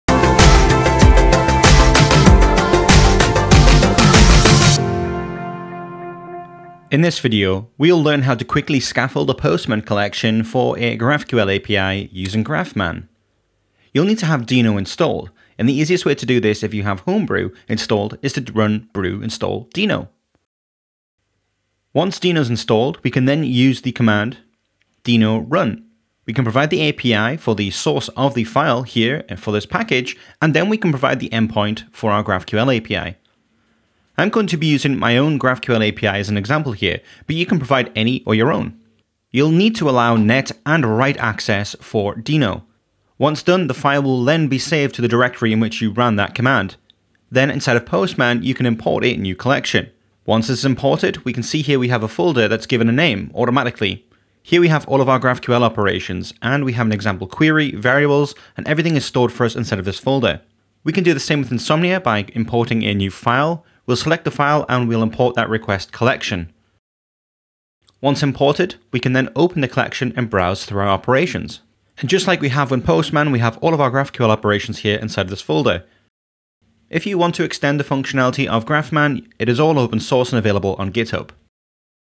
GraphQL to Postman collection with GraphMan
GraphMan is a great utility that runs via Deno to automatically convert your GraphQL endpoint to a collection that can be imported to Postman, and Insomnia.

GraphMan runs over Deno, and introspects your schema to get all available operations, fields, arguments, variables, and more.